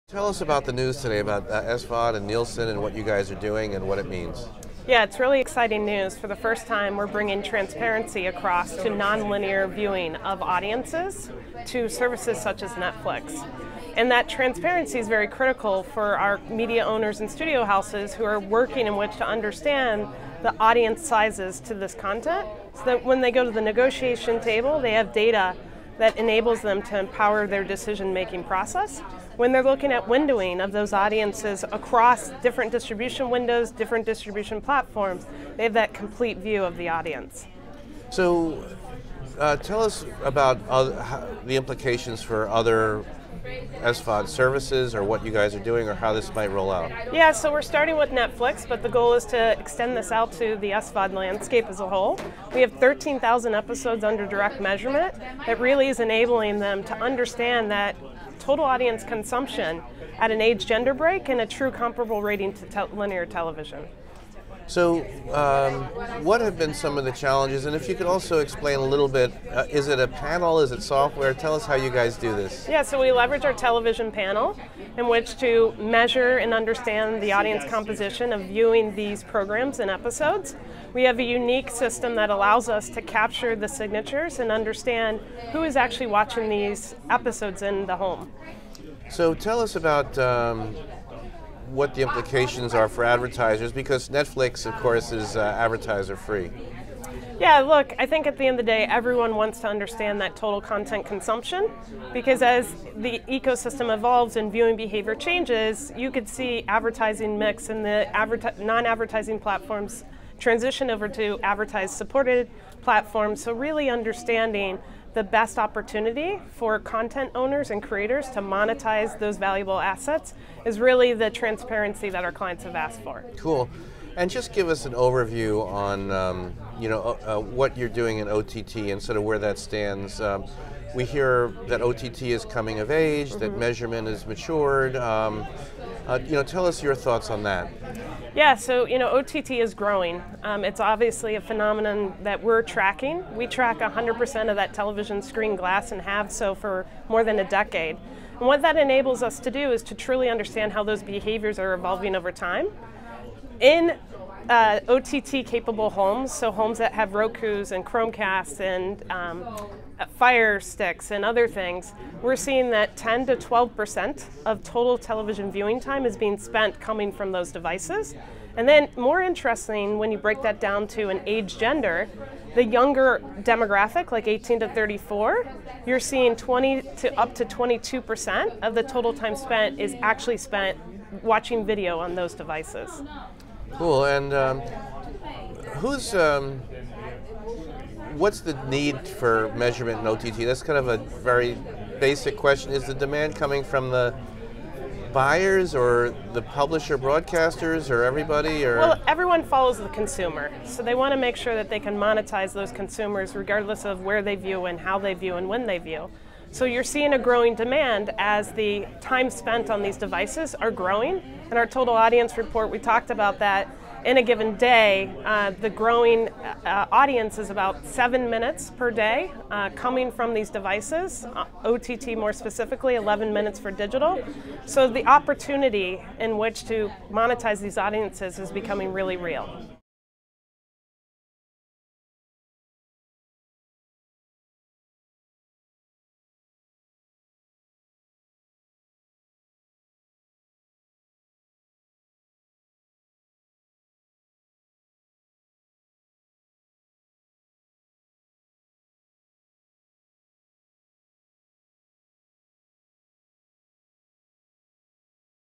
Nielsen Enables Easier Measurement Of Netflix Consumption: Abcarian
Not that Netflix is open to advertisers yet - but, if it ever is, those advertisers may credit 2017 as the year when the building blocks of third-party Netflix measurement were put in place.

Nielsen just switched on measurement for 13,000 streaming Netflix shows using TV-style ratings based on age and gender - the same thing advertisers have used to buy linear TV ads for decades.

Nielsen actually began measuring streaming content in 2014 via an opt-in service. But, until now, clients needed to submit their own audio signatures for the shows they wanted to measure. Now Nielsen has enhanced the system with signatures for all those Netflix shows as a syndicated service inside its SVOD Content Ratings.

"We are starting with Netflix, but the goal is to extend this out to the SVOD landscape as a whole," says Nielsen's product management SVP Kelly Abcarian in this video interview with Beet.TV. "It really is enabling them to understand that total audience consumption at an age/gender break and a true comparable rating to linear television."

Today, it seems like Nielsen's best customer base for the product will be TV show makers themselves, as they try to better understand their work's consumption and gain leverage for carriage negotiation.

"For the first time, we are bringing transparency across to non-linear viewing of audiences, to services such as Netflix," Abcarian says.

"That transparency is very critical for our media owners and studio houses who are working and want to understand the audience sizes to this content, so that, when they go to the negotiation table, they have data that enables them to empower their decision-making process."

But, if Netflix ever did switch on advertising, it seems like a solution would be in place to help the trading of ads there, too.

Ampere Analysis analyst Richard Broughton has previously told Beet.TV that Netflix could make up to $8bn from a full switch to advertising.